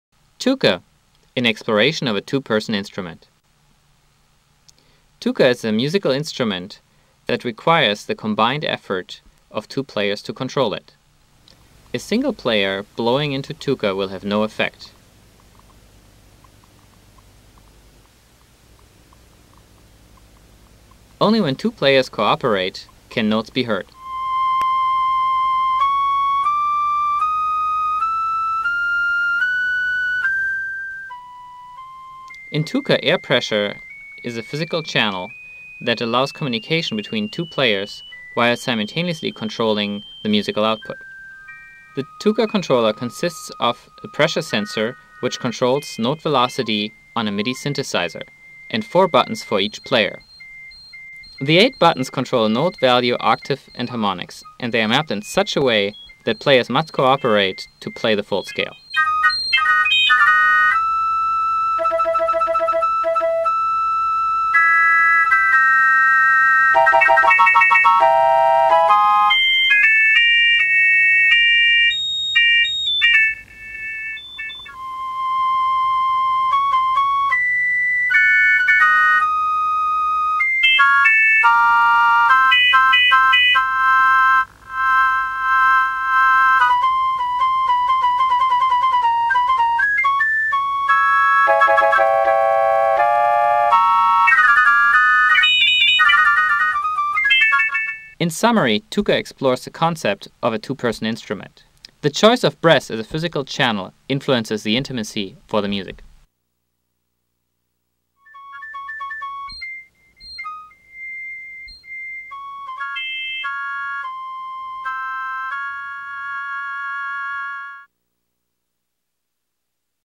Tooka: a Two Person Flute - Video of 2004 version of Tooka being played
We describe three new music controllers, each designed to be played by two players. As the intimacy between two people increases so does their ability to anticipate and predict the other's actions. We hypothesize that this intimacy between two people can be used as a basis for new controllers for musical expression. Looking at ways people communicate non-verbally, we are developing three new instruments based on different communication channels. The Tooka is a hollow tube with a pressure sensor and buttons for each player. Players place opposite ends in their mouths and modulate the pressure in the tube with their tongues and lungs, controlling sound. Coordinated button presses control the music as well. The Pushka , yet to be built, is a semi-rigid rod with strain gauges and position sensors to track the rod's position. Each player holds opposite ends of the rod and manipulates it together. Bend, end point position, velocity and acceleration and torque are mapped to musical parameters. The Pullka, yet to be built, is simply a string attached at both ends with two bridges. Tension is measured with strain gauges. Players manipulate the string tension at each end together to modulate sound. We are looking at different musical mappings appropriate for two players.

This project is open for 496 projects to continue to improve Tooka and/or make Pushka and Pullka. Musical knowledge is an asset.